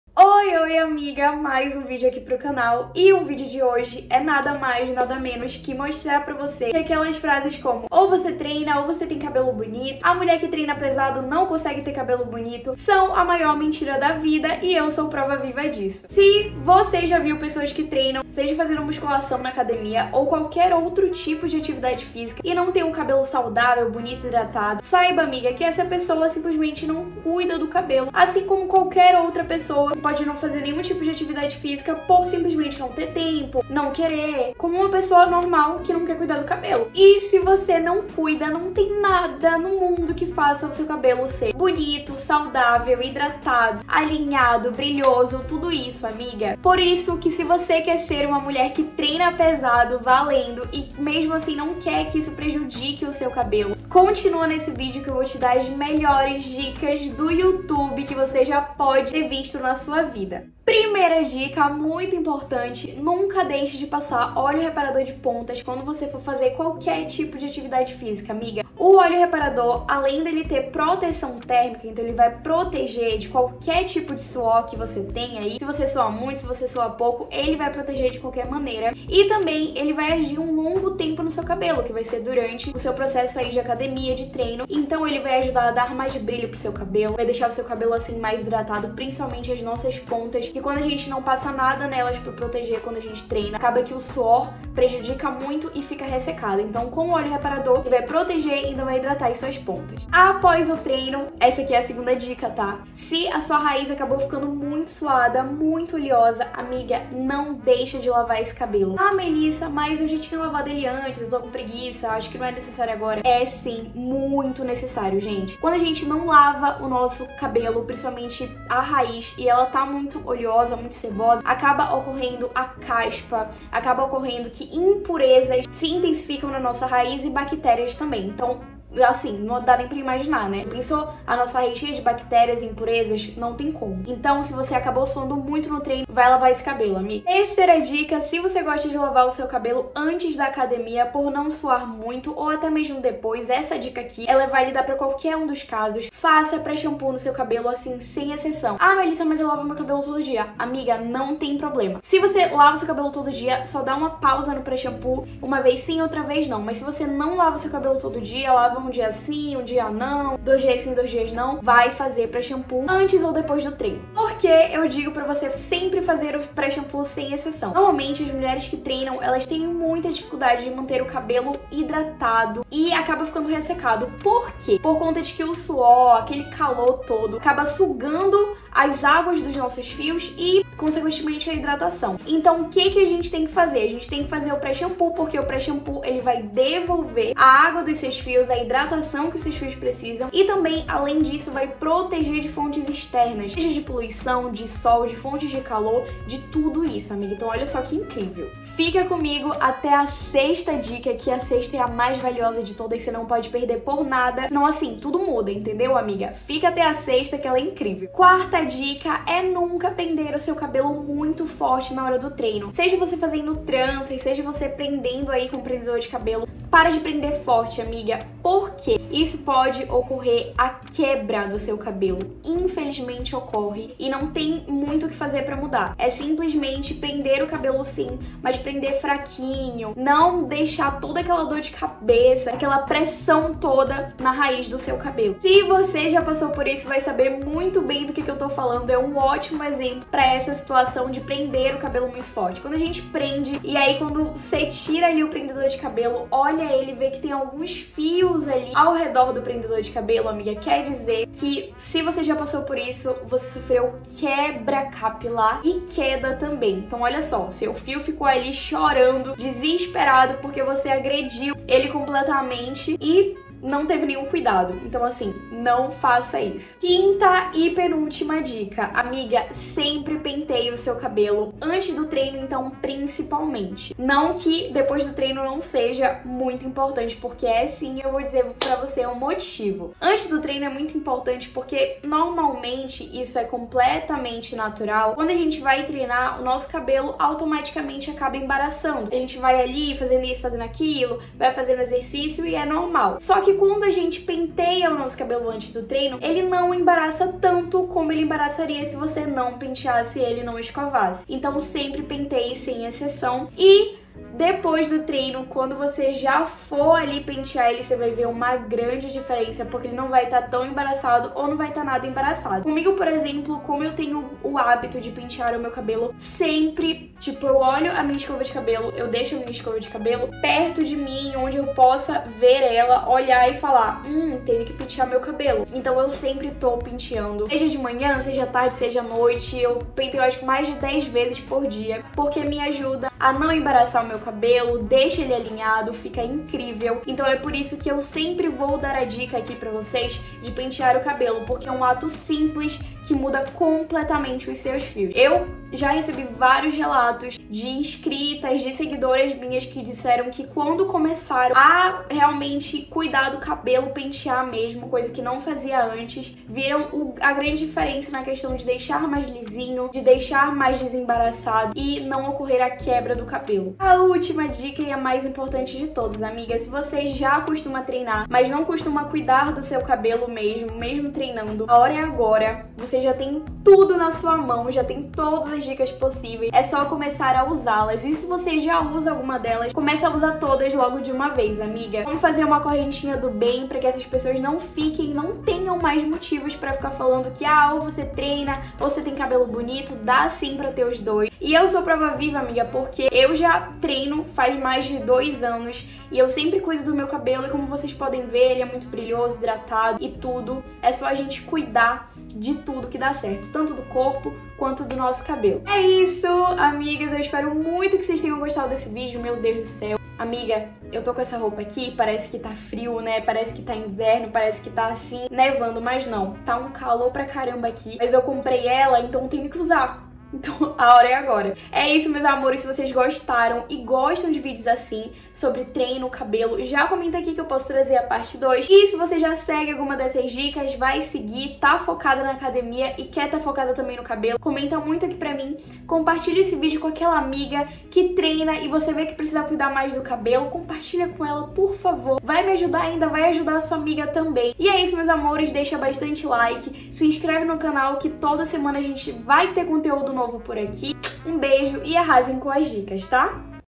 AS MELHORES DICAS CAPILARES PRA QUEM TREINA 💪🏼💆🏻‍♀️
Nesse vídeo, você vai aprender o segredo pra ter o cabelo hidratado, saudável e lindo mesmo treinando pesado e sim, pode ter certeza que isso é possível, amiga! Sou prova viva disso e vou passar pra você todas as dicas e tudo que faço pra manter as madeixas brilhosas e maravilhosas

Me siga no Instagram para mais dicas de beleza em geral
› antunessmel1Resultados da WebMelissa Antunes (@antunessmel1) • Instagram photos and videos